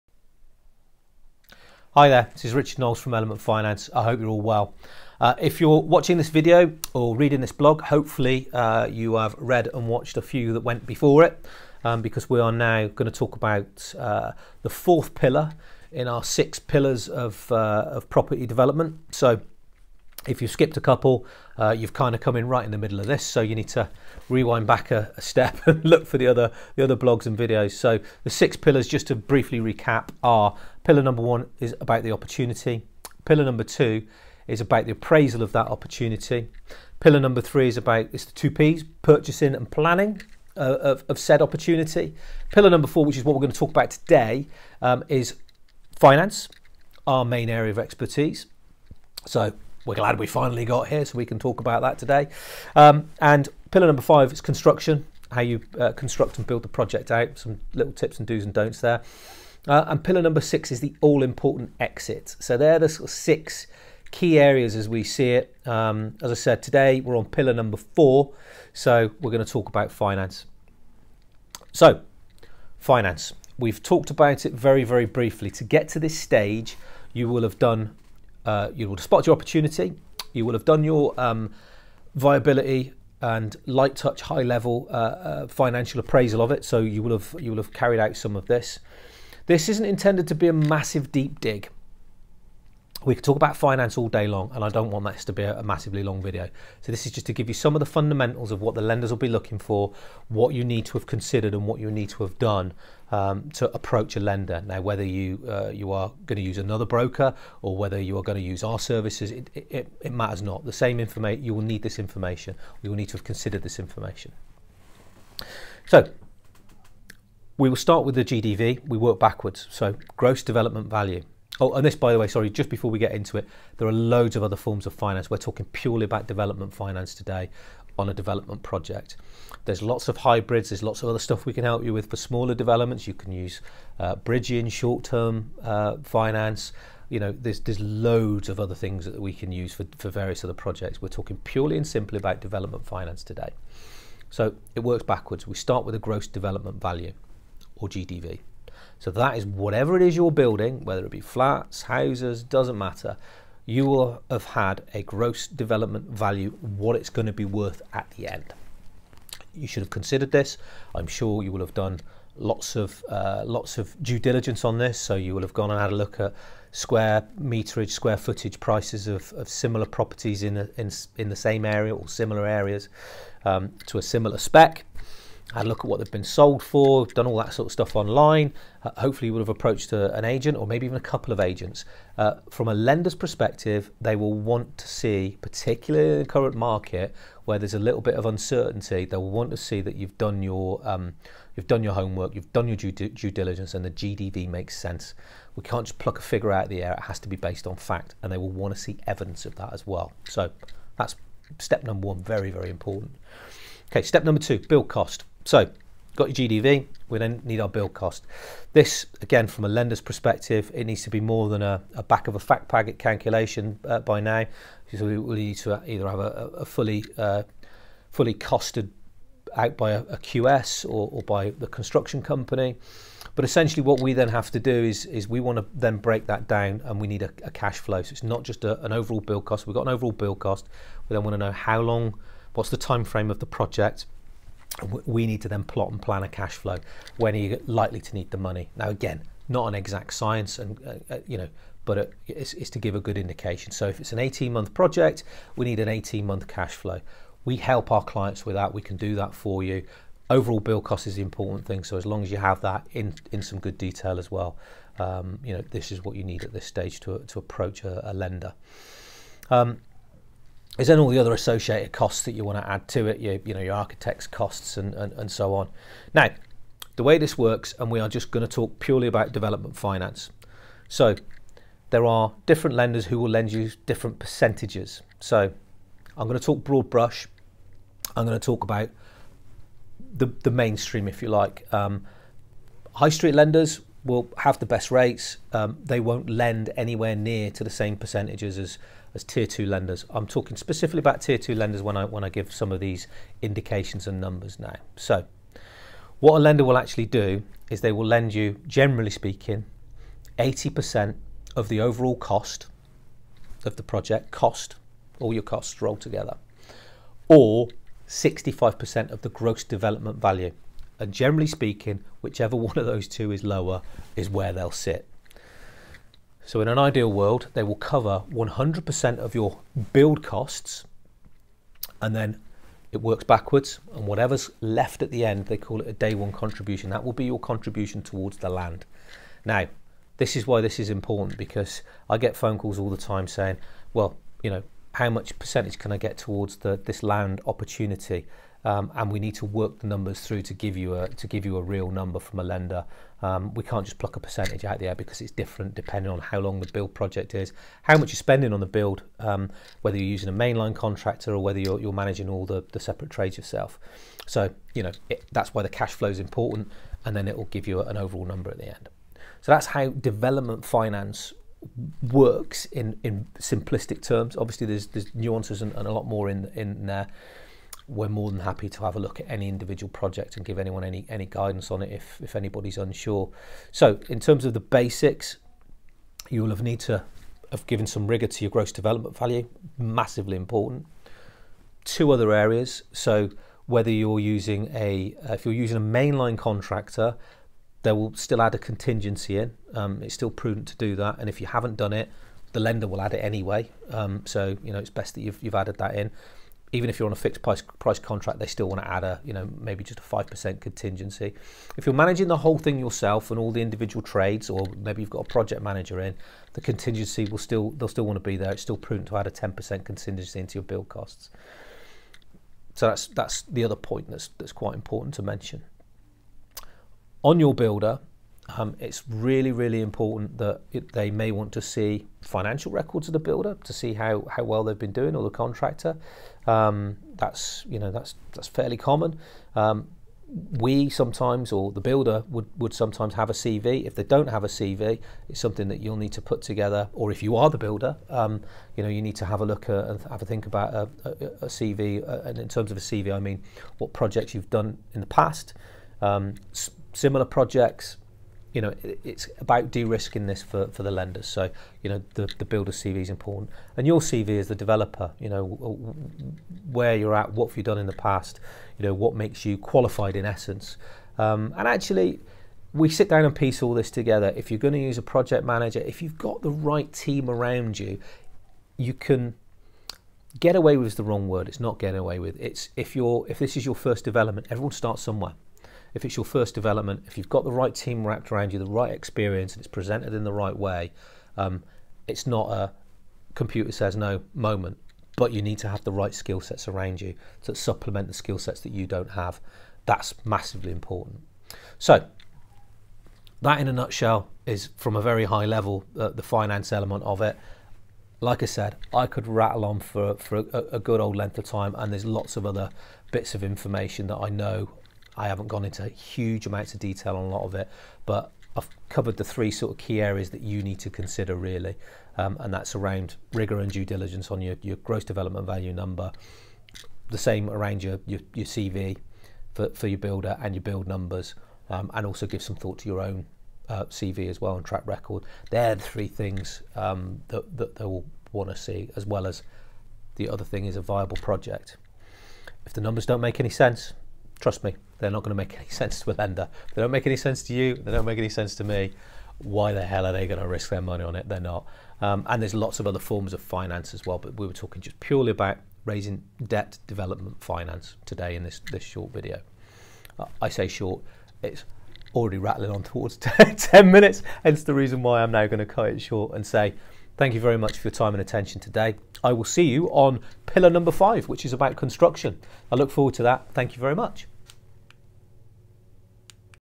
Pillar4 Finance
Taken from the 6 Pillars of property development this video is all about how you Finance your property development project.